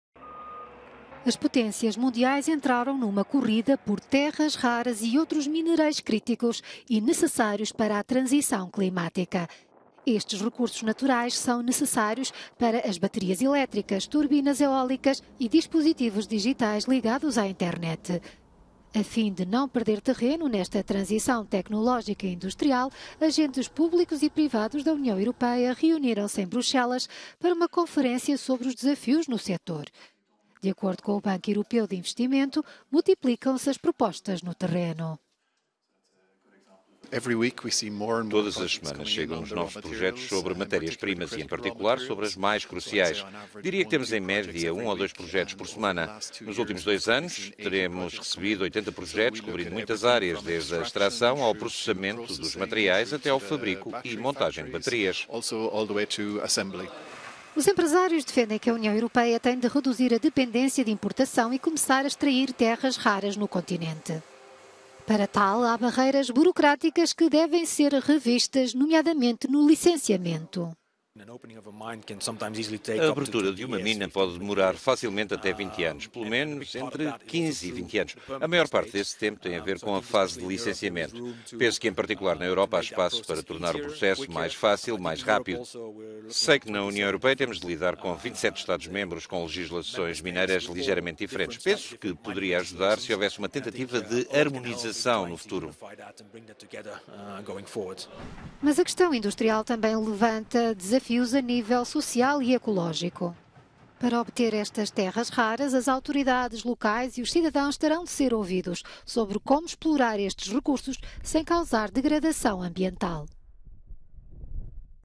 Europa precisa de atualizar legislação na corrida por terras raras
Os empresarios defendem que a UE tem de reduzir a dependência de importação e começar a extrair terras raras no continente. Para tal, há barreias burocrátricas que devem ser revistas, nomeadamente no licenciamento.